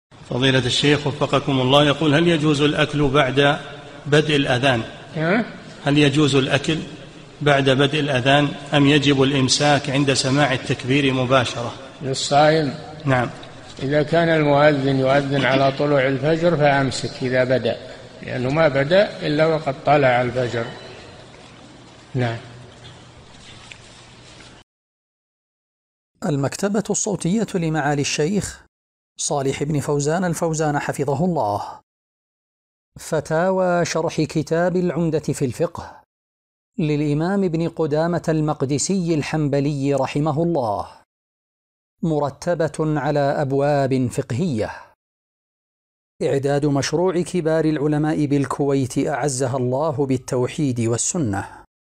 [440 -3022] هل يجوز الأكل أثناء الأذان أم يجب الإمساك عند سماع التكبير مباشرة للصائم؟
-"[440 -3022] السؤال: هل يجوز الأكل بعد بدء الأذان أم يجب الإمساك عند سماع التكبير مباشرة للصائم؟
الجَوَابُ: نعم، إذا كان المؤذن يؤذن على طلوع الفجر فأمسك إذا بدأ، لأنه ما بدأ إلا وقد طلع الفجر، نعم.
للاشتراك في قناة الشيخ صالح الفوزان

للاستفسار والإقتراحات والدعم
رئيس الفريق
لمشروع كبار العلماء